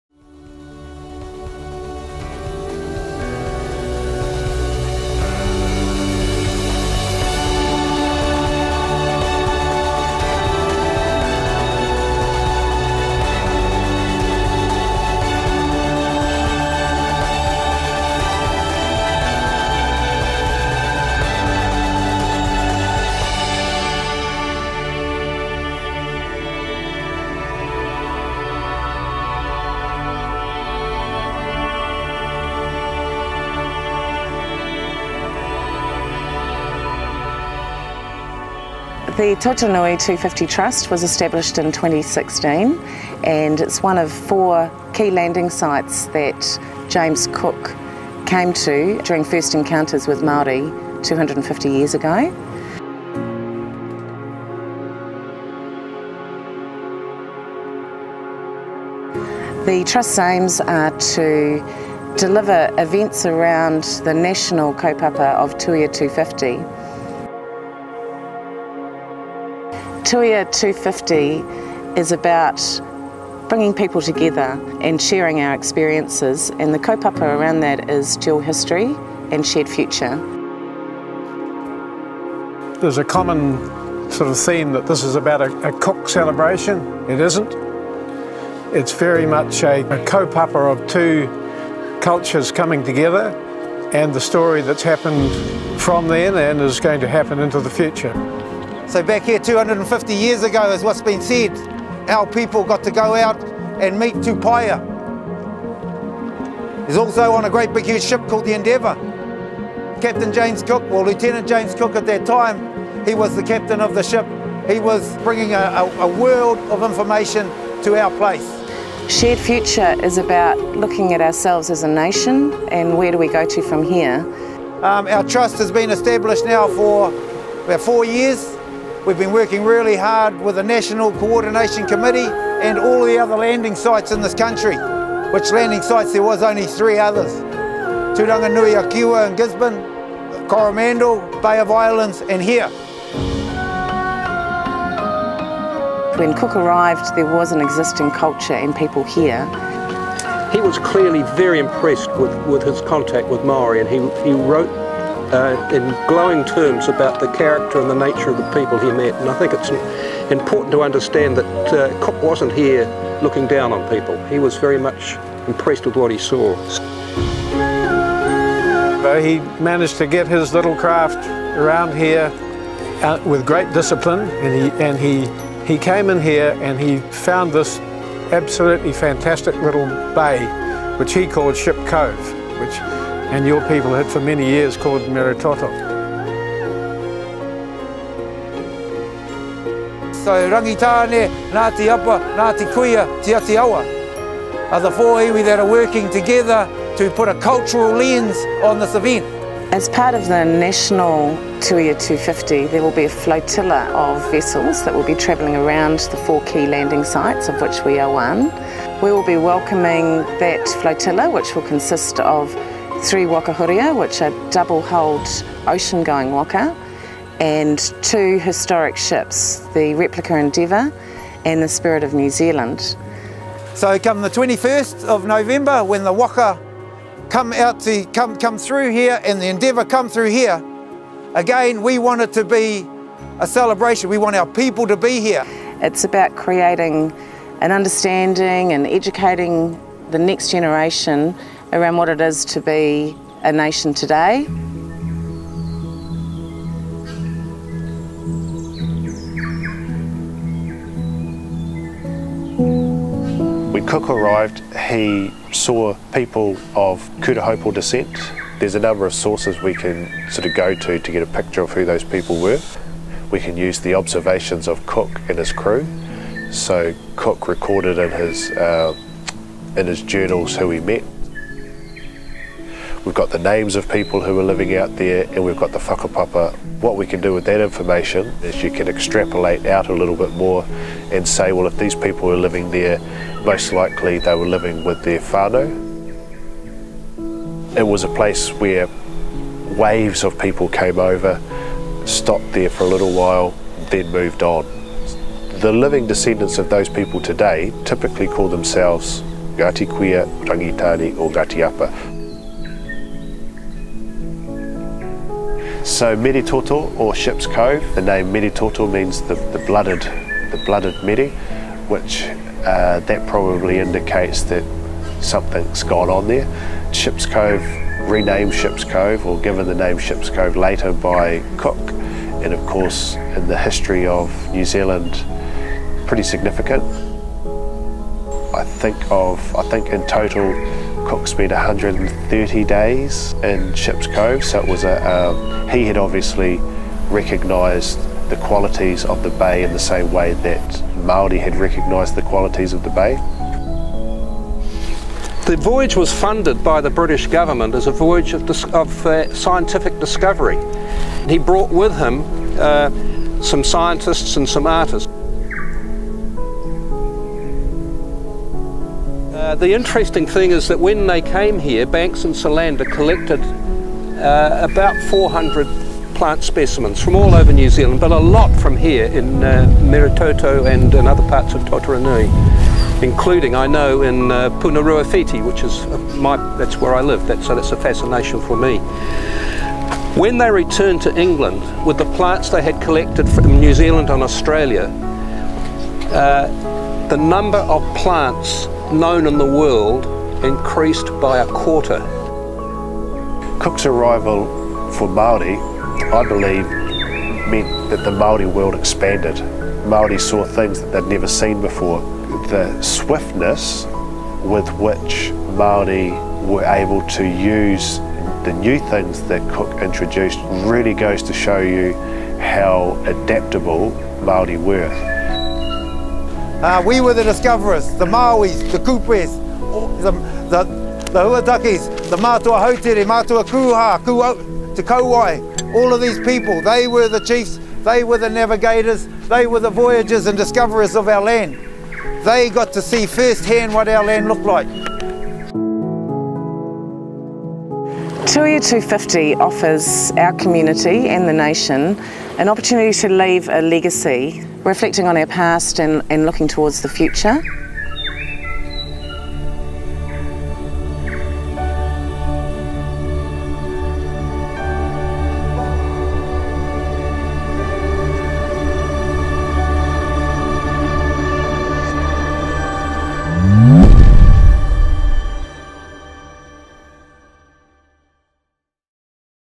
First Encounters Meretoto/Ship Cove
Tuia 250 is about bringing people together and sharing our experiences.  A dual heritage and shared future.

It's about looking at ourselves as a nation and where we go from here.